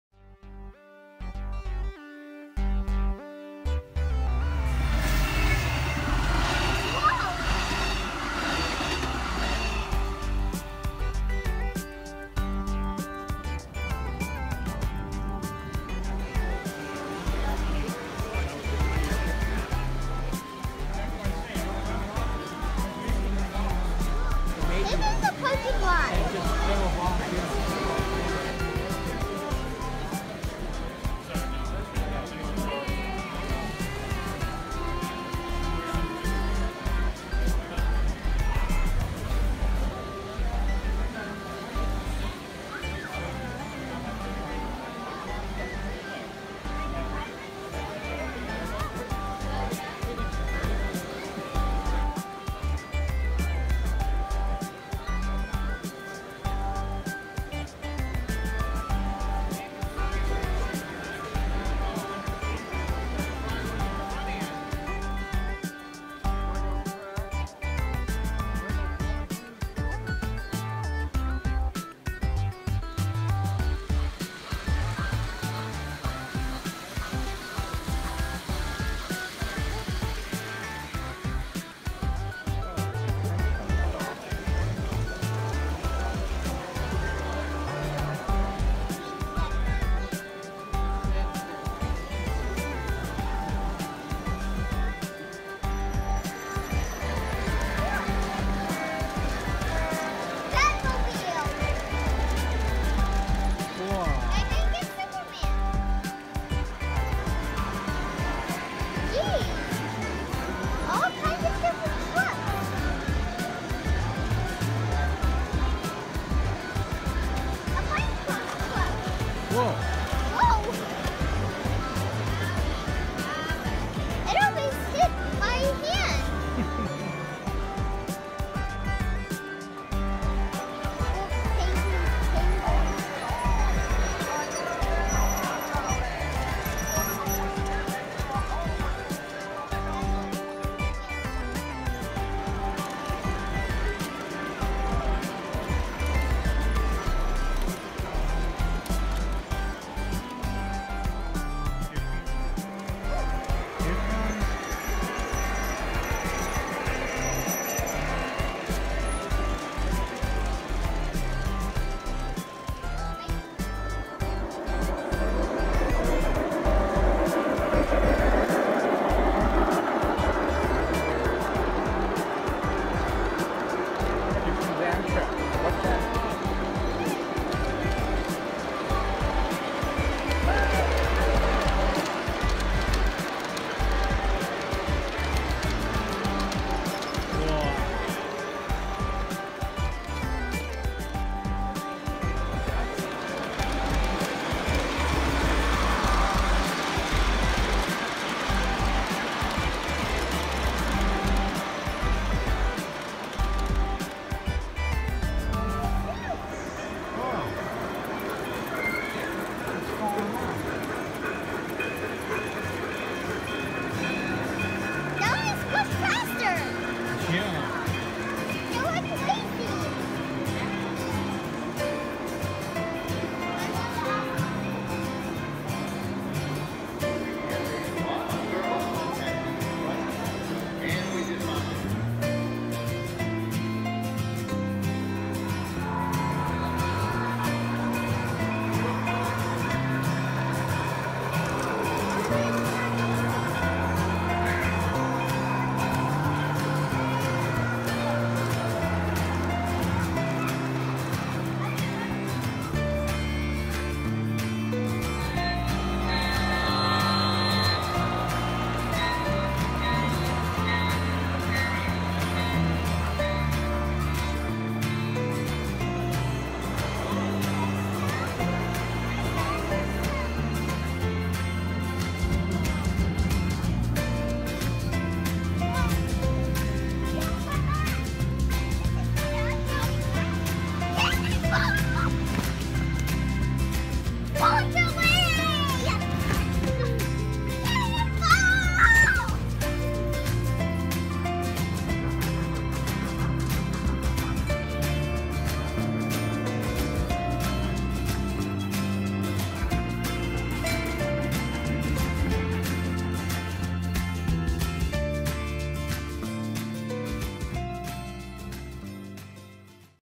Great Train Show 2017 San Jose
B's trip to the Santa Clara County Fair Grounds to see the Great Train Show, he had a blast!